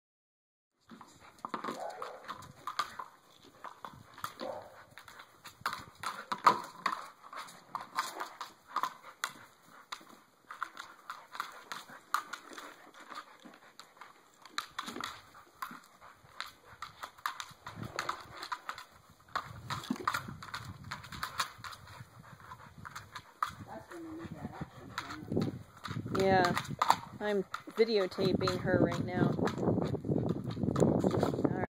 A Dog and Her Ball
Chloe is playing with her big red ball